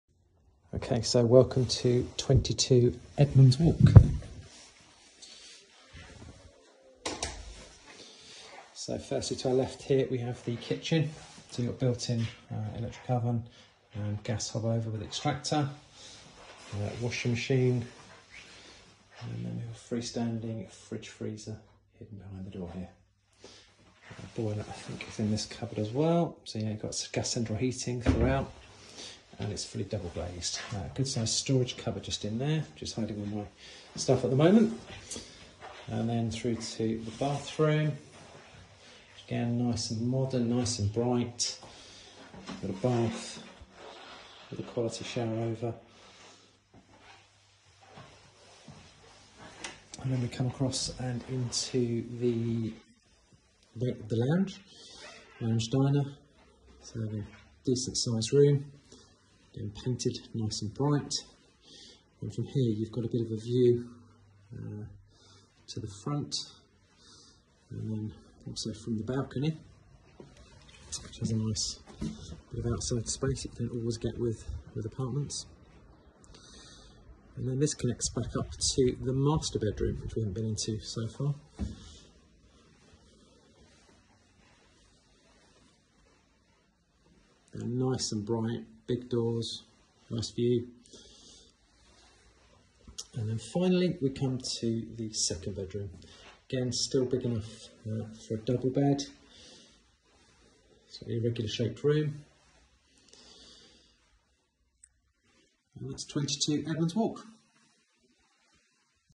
22 Edmonds Walk - Video Viewing
Bright upper floor 2 bedroom flat is situated in the popular residential area of Torre Marine - Available Now